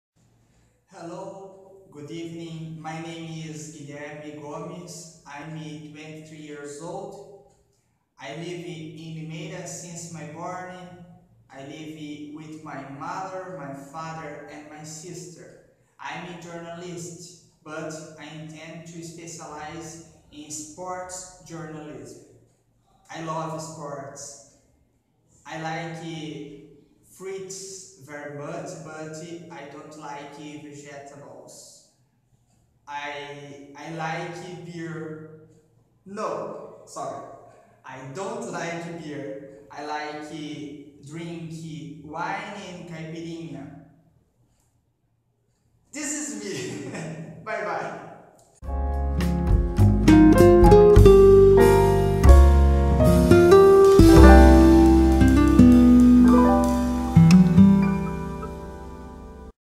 Guilherme Gomes | Exercício Prático 1 | English You
Exercício prático 1 feito pelos alunos do módulo 1 do curso de Inglês na English You.